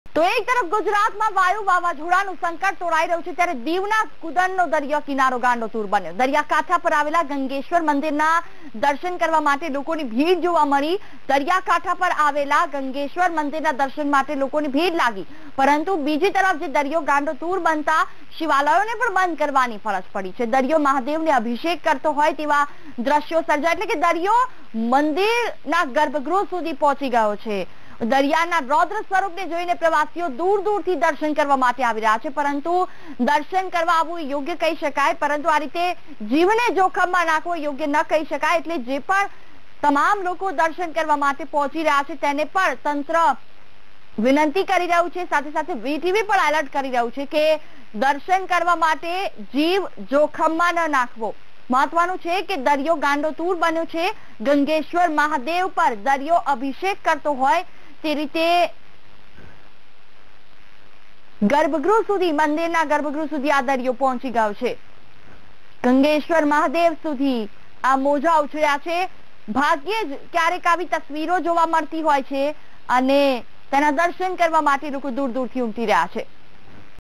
Diu ના કૂદમનો દરિયો બન્યો ગાંડો તુર; દરિયાની રોદ્ર સ્વરૂપને જોઇ પ્રવાસીઓની લાગી ભીડ | Vtv
Diu ના કૂદમનો દરિયો બન્યો ગાંડો તુર; દરિયાની રોદ્ર સ્વરૂપને જોઇ પ્રવાસીઓની લાગી ભીડ